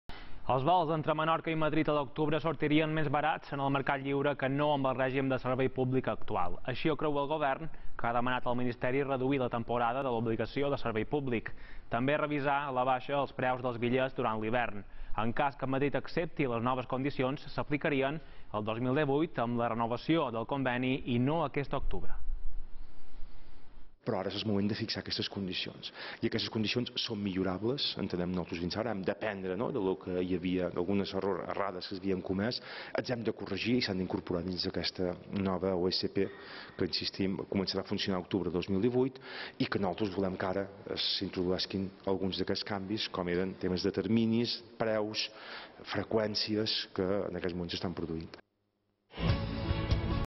El Govern demana a Madrid reduir la temporada de l’OSP entre Menorca i Madrid
Els vols entre Menorca i Madrid a l’octubre sortirien més barats en el mercat lliure que no amb el règim de servei públic actual. Així ho creu el Govern, que ha demanat al Ministeri reduir la temporada de l’Obligació de Servei Públic. També, revisar a la baixa els preus dels billets durant l’hivern. En cas que Madrid accepti les noves condicions, s’aplicarien el 2018, amb la renovació del conveni, i no aquest octubre.